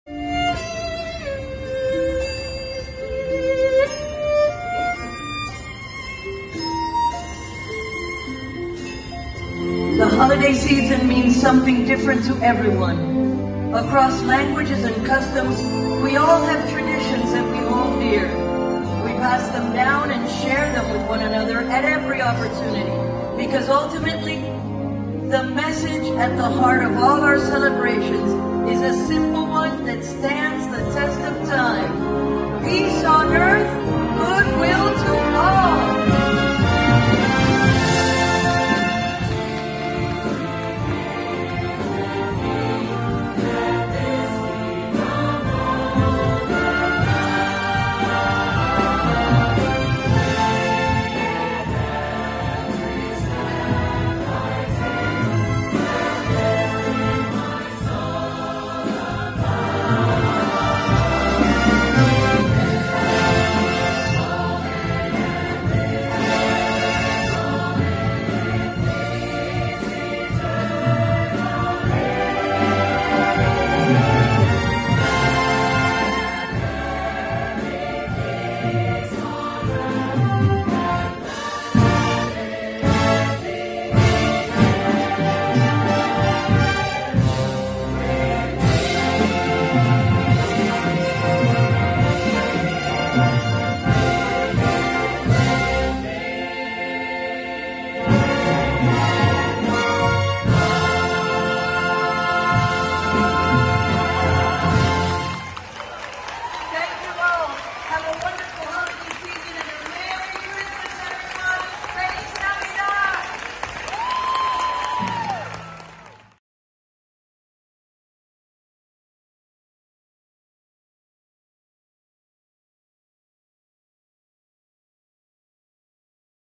Gloria Estefan narrating EPCOT's Candlelight Processional | December 13, 2022
"What a beautiful night at Epcot Center! Thank you everyone who came out tonight! Happy Holidays, love and peace to ALL!!" (Gloria Estefan)